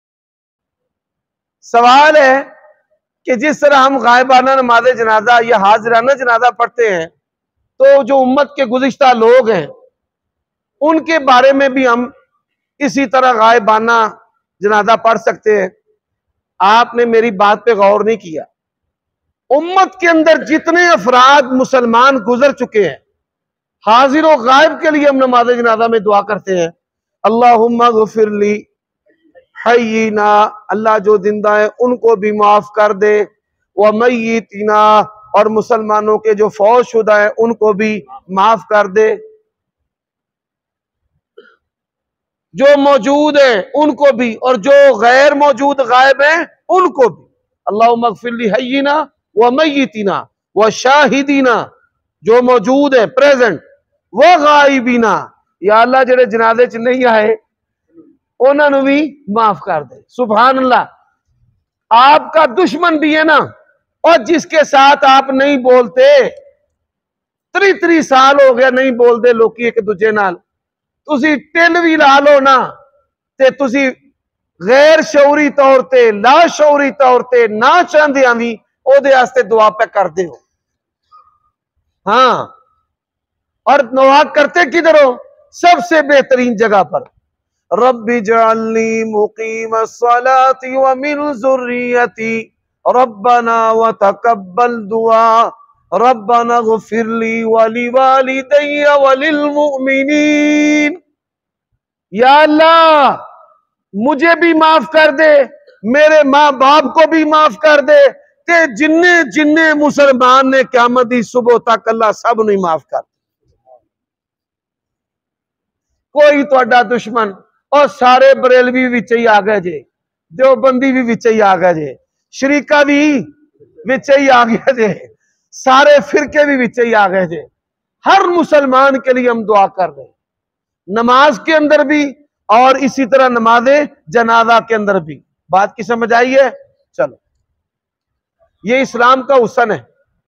Rabi Ul Awal Best Clip | Mai Brelvio Ky Laye Dua Krta Hn | Woh Bi Krty Hai | Hafiz Umar Siddique
بریلویوں کیلئے دعائیں
🎙️مفتی حافظ محمد عمر صدیق حفظہ اللہ تعالیٰ

Ap Ko Hamary Channel Par
Hafiz Muhammad Umar Siddique
Sahib Ka Har Khutbha Jumma Mil Jay Ga. !